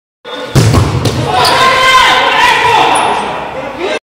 Defesa de MK27 Euenio Mukiado Fagundes - Amigos de Danilo FC Campeão Festival Futsal Kadosh 2021
Amigos de Danilo FC Campeão Festival Futsal Kadosh 2021
Vídeo de Danilo Ornelas